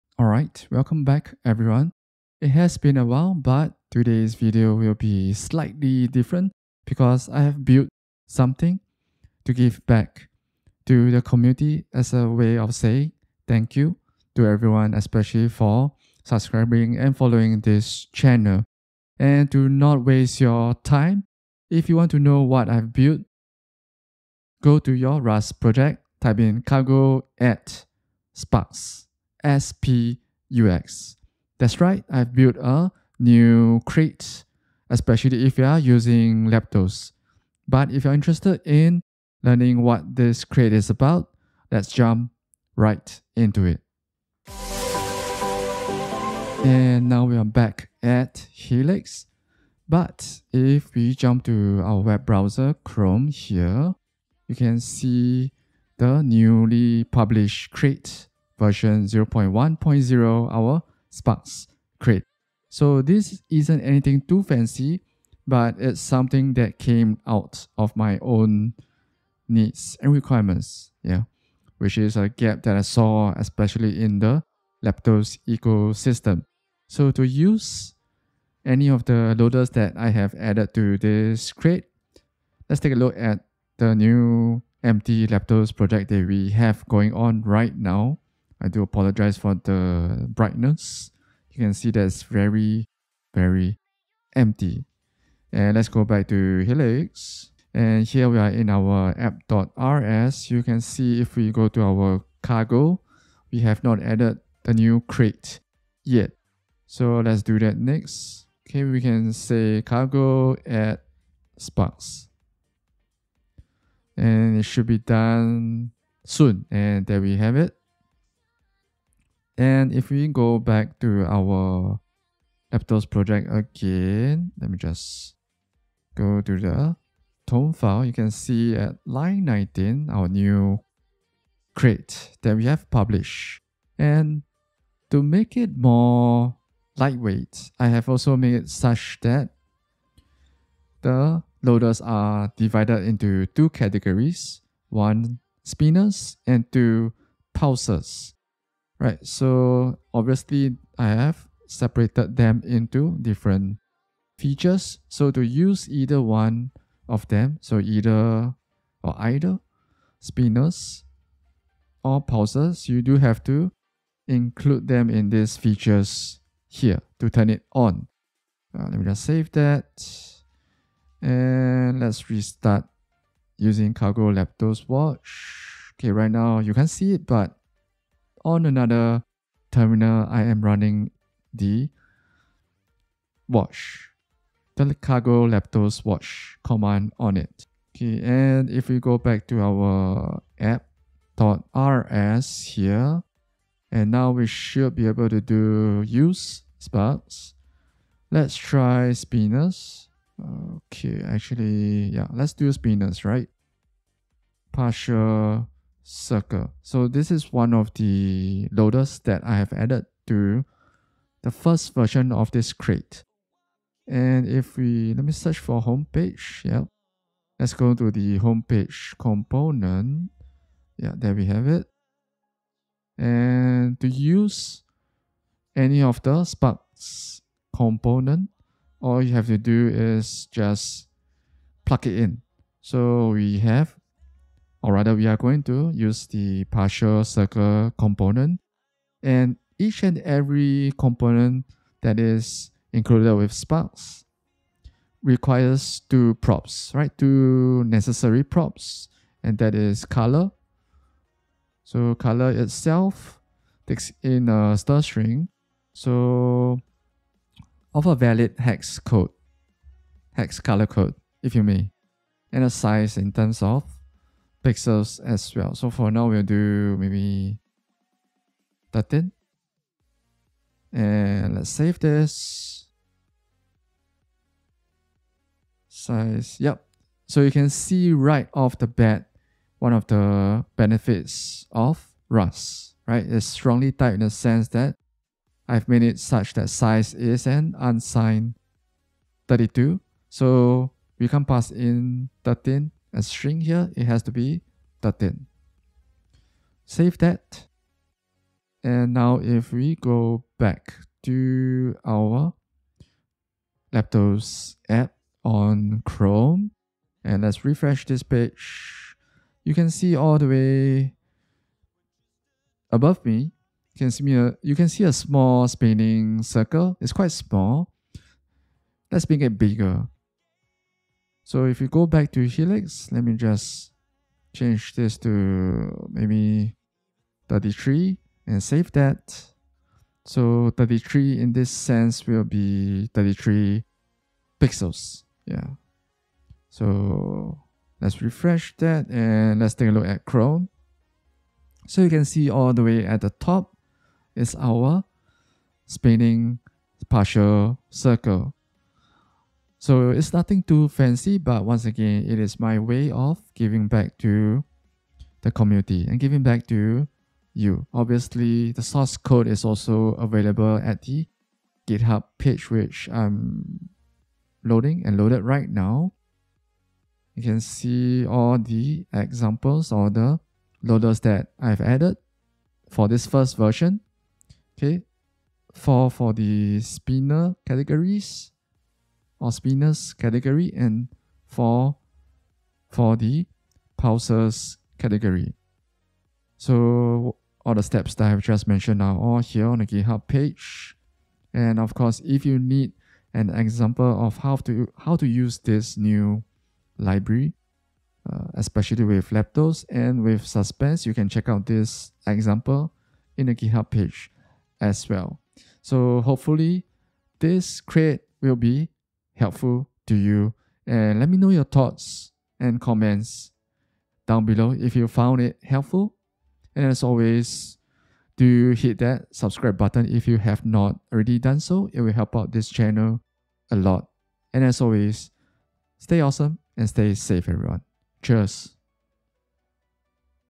Giving Back to the Rust Community?
It's finally time to build something and give back to the Rust (and Leptos) community! Let's jump in to see what I have built!